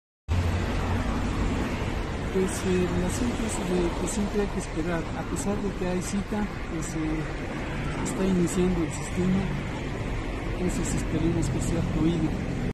Conductor acude a la Verificación Vehicular y a pesar de tener cita le han solicitado esperar
Conductores como el señor Leonardo Macías, quien acude por primera vez, se refiere que a pesar de tener cita le han solicitado esperar "en lo que inicia el sistema". Confió en que la Verificación Vehicular sea rápida y fluida.